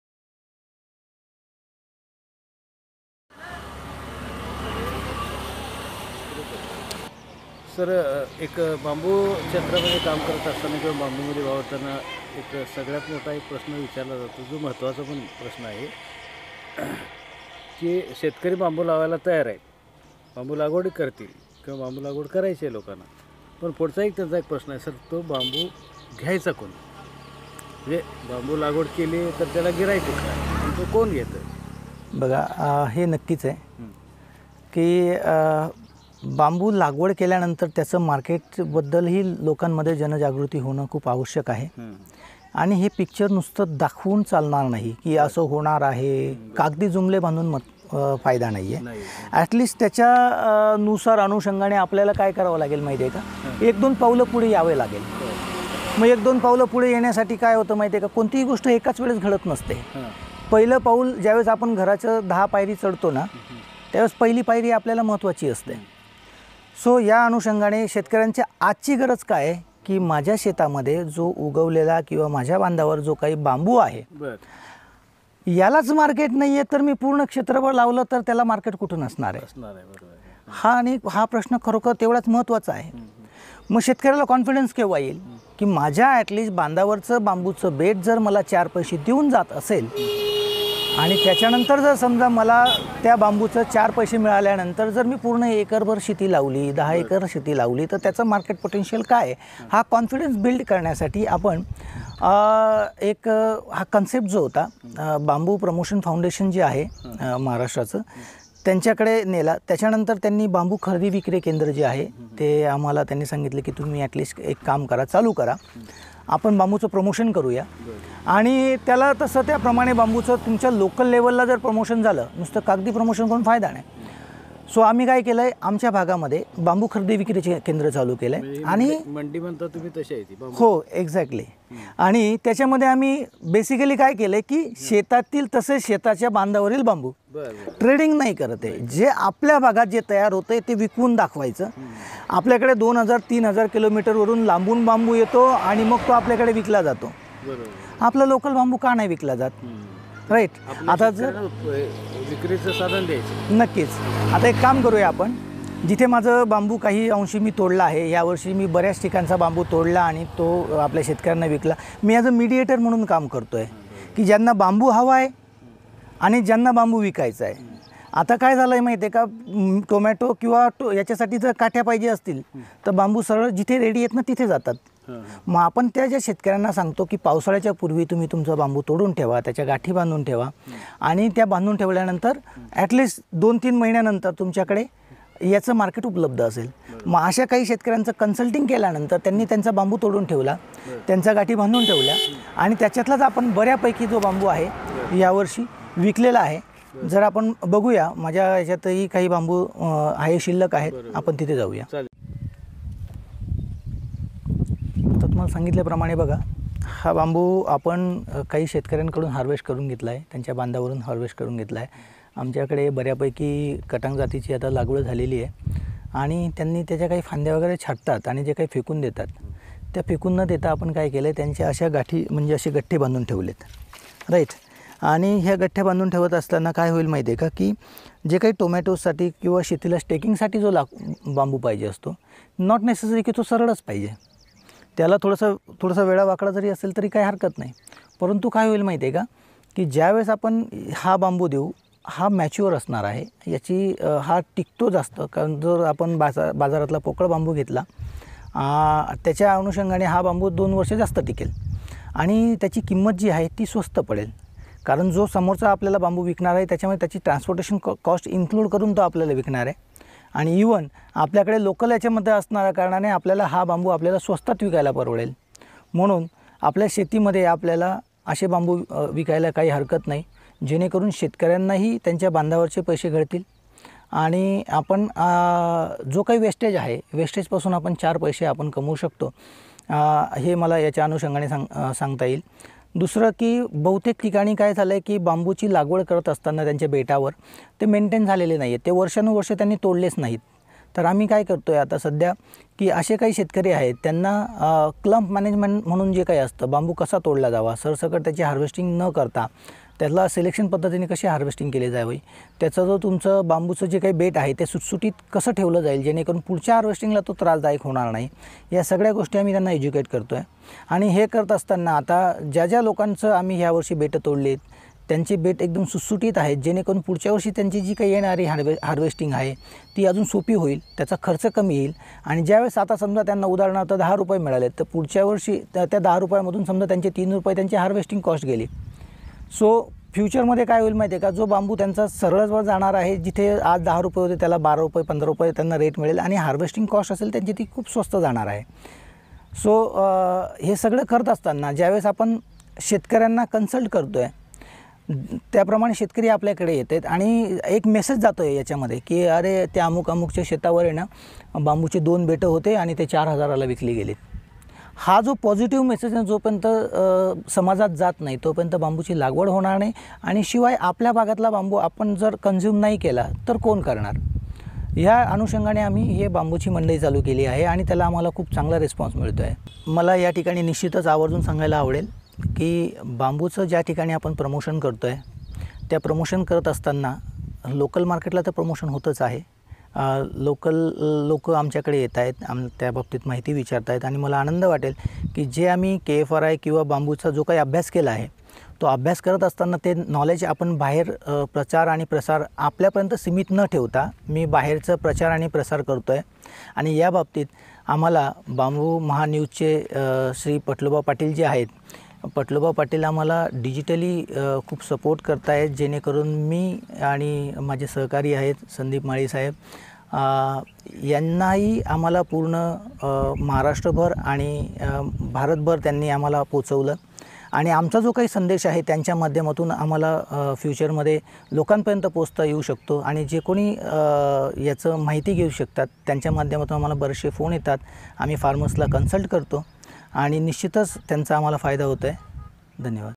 बांबू मंडई शेतकऱ्यांसाठी उपयुक्त असल्याबाबत उमेश सोनार यांची पाटलोबा पाटील यांनी घेतलेली मुलाखत
Bamboomaha News
creator - Pataloba Patil